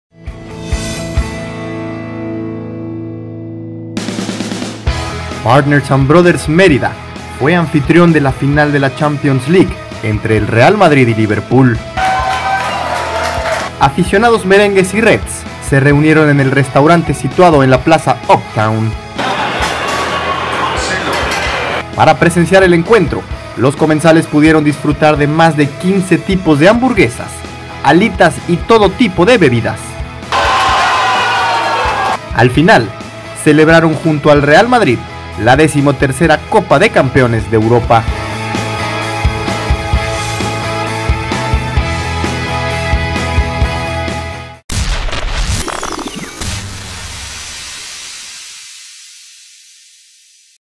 Partners and Brothers Merida
Aficionados del Real Madrid y Liverpool disfrutaron la final de la Champions League en Partners and Brothers Mérida de la plaza Uptown acompañado de Hamburguesas y alitas.